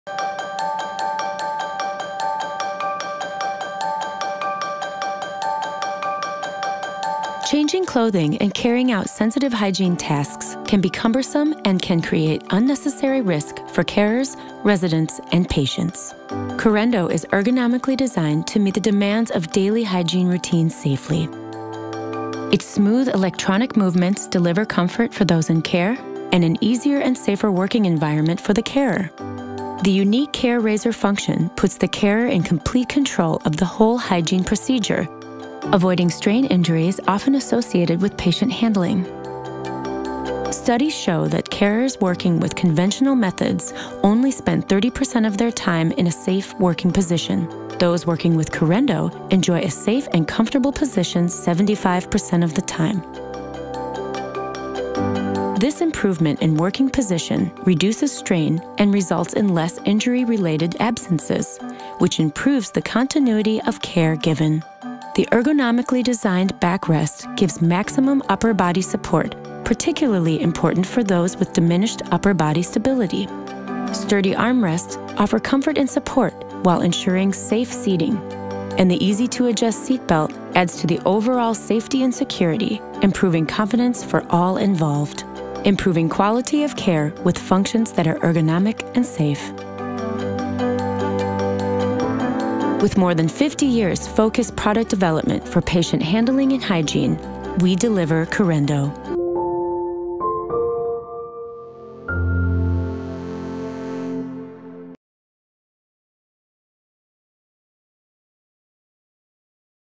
Carendo hygiene and showering chair for ergonomics and safety | Arjo Global
The Carendo improves efficiency of the care, as it enables a single caregiver to perform the entire showering procedure, as well as a wide range of other everyday hygiene tasks. The high level of support provided by the Carendo results in streamlined routines that offer considerable time savings compared to traditional methods.